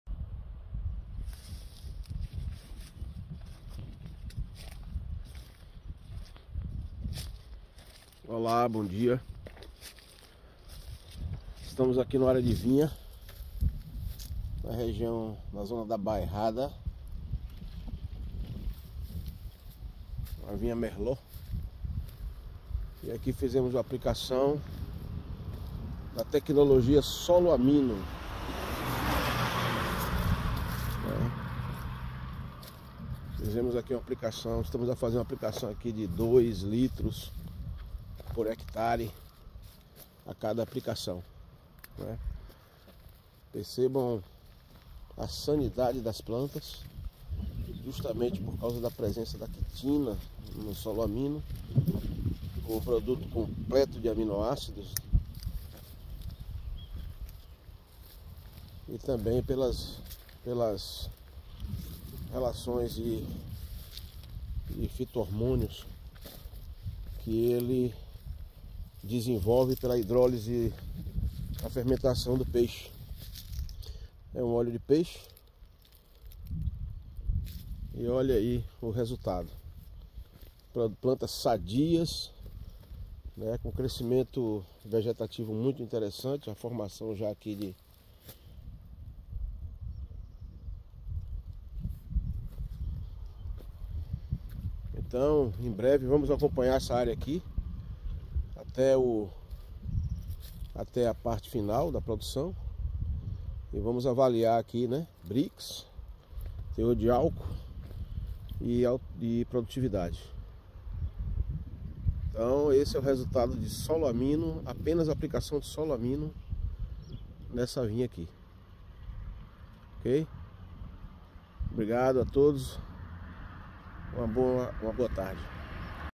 Solöamino®, regeneramos cultivos regenerando suelos. Aceleramos procesos de transición regenerativa.
En SOLÖAMINO® trabajamos la nutrición vegetal a través de la regeneración del suelo.
Aportando materia orgánica de descomposición rápida conjuntamente con más de 30 millones de microorganismos por ml, logramos recuperar y regenerar suelos improductivos, cansados o con poca actividad microbiana.
Solöamino® libera nutrientes indisponibles del suelo, regenera y reactiva la vida microbiana de la rizosfera.
Conseguimos cosechas con hasta 100% de diferencia con el padrón medio.
Nos especializamos tanto en la regeneración de cultivos improductivos como de sacar el máximo rendimiento en cultivos de punta.
Solöamino® trae vida a su suelo, ahorrando tiempo y asegurando su alta productividad.
Regeneramos cultivos regenerando suelos.

SOLÖAMINO®, Se trata de un complejo de nutrientes provenientes del hidrolizado enzimático de pescados.
Se recomienda su utilización en todo tipo de cultura, especialmente en épocas de estrés o pos-traumáticas.
SOLÖAMINO® es un Abono Orgánico Líquido.

18 tipos de aminoácidos de proteína animal.
Quitina natural.
Microorganismos.
Todos los macro y micro nutrientes orgánicos.
35% de materia orgánica.
12% de proteina.
17% de carbono orgánico.
12% aceite natural de pescado.
Libre de patógenos.

Hidrolizado de pescado - pescado en agricultura - abono de pescado - mejor abono del mundo